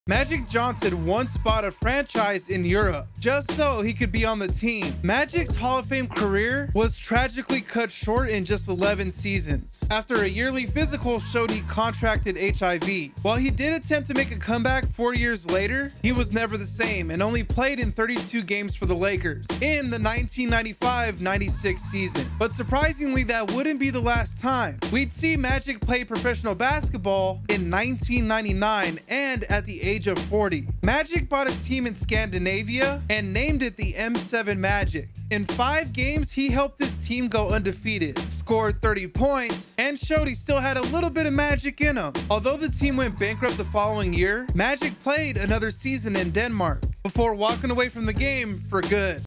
The Time Magic Johnson DOMINATED In Europe 💪 I Clutch
Los Angeles Lakers point guard Magic Johnson’s NBA career was cut short due to testing positive for HIV. While his game never recovered, his passion never wavered. And he showed flashes of brilliance a few years later when he played for multiple teams in Europe.